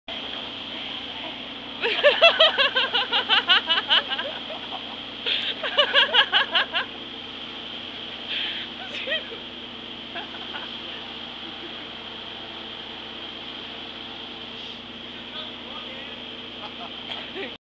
waiting for Dir en grey 11/10/2008
This is my friend Slaytanic running and screaming "Viva Mexico" but you can't hear him because of my obnoxious laughing... (Ugh I sound retarded) but it is still funny!

We were trying to amuse ourselves after the concert at Ramshead while waiting for the band to come out to their tour bus.